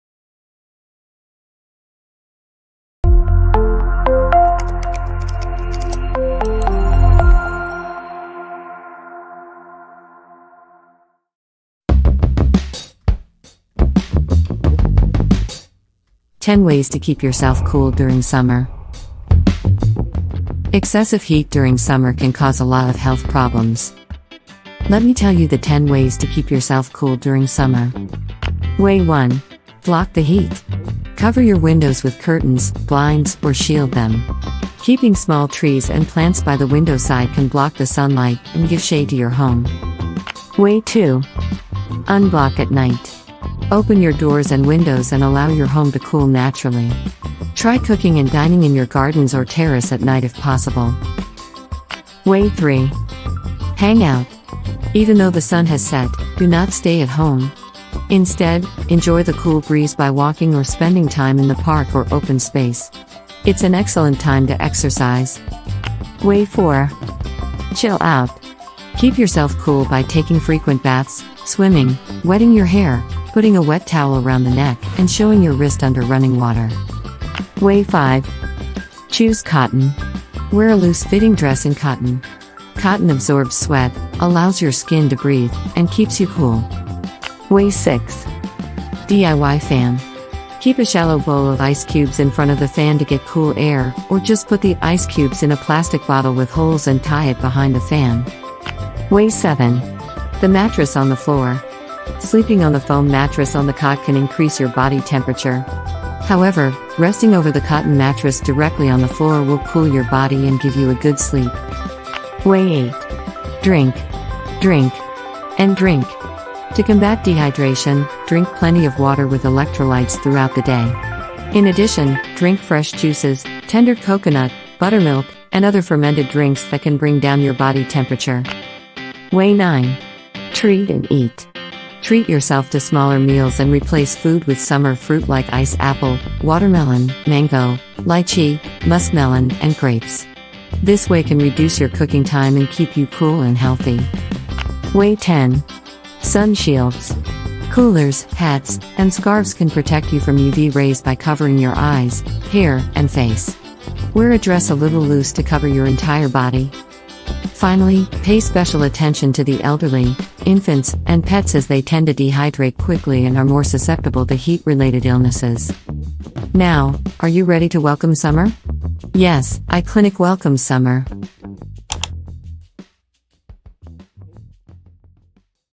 10 ways to keep yourself cool during summer | iCliniq
Watch the video and let's have a cool and funfilled summer from now on